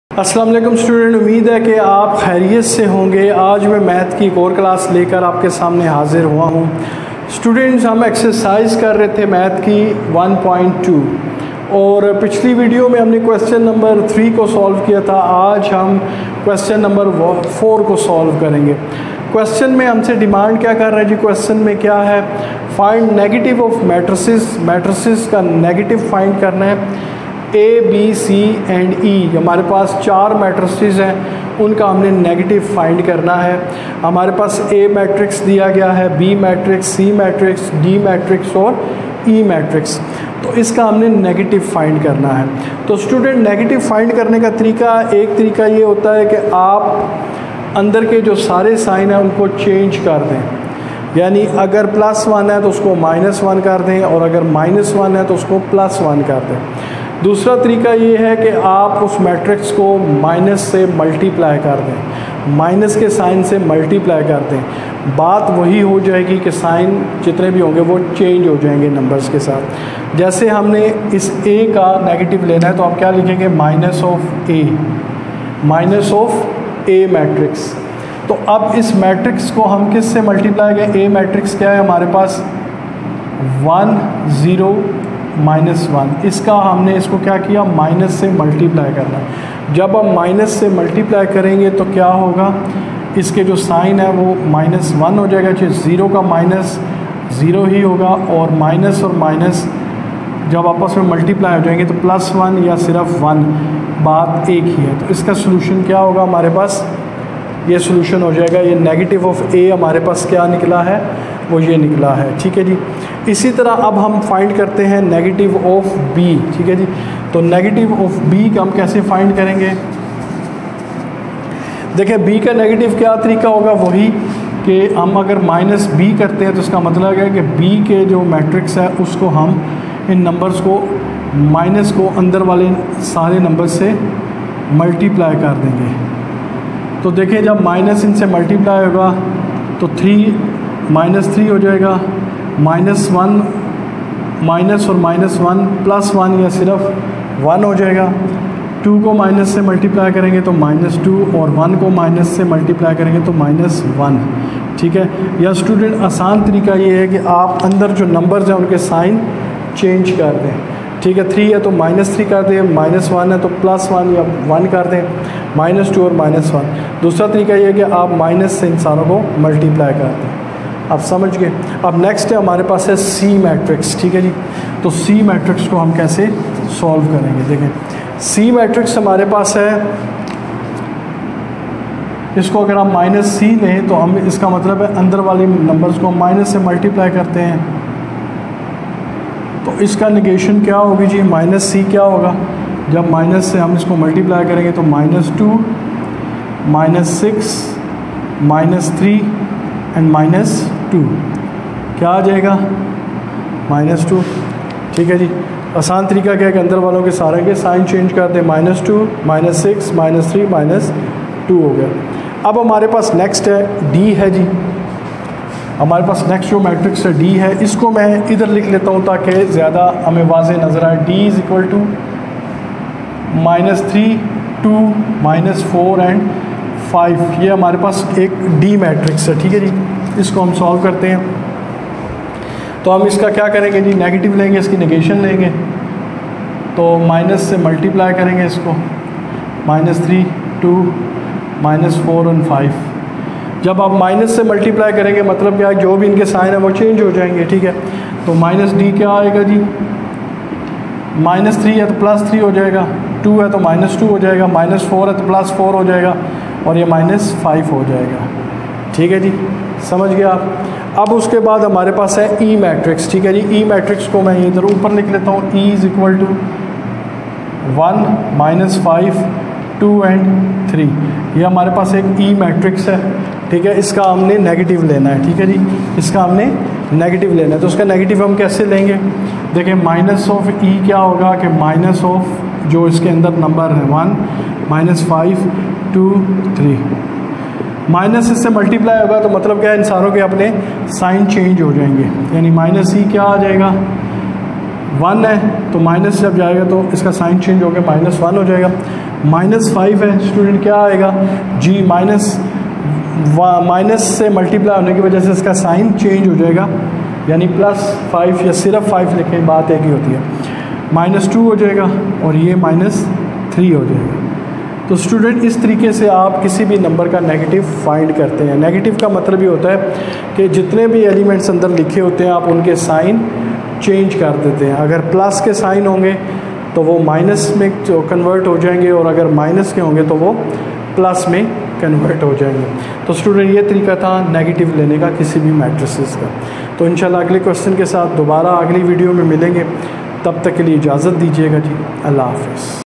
Matrices & Determinants Exercise 1.2 (Q.No. 4) Maths, Unit: 1,Class: 9th by Leaders Knowledge Hub.
In this video I will solve the Q.No. 4 of Exercise 1.2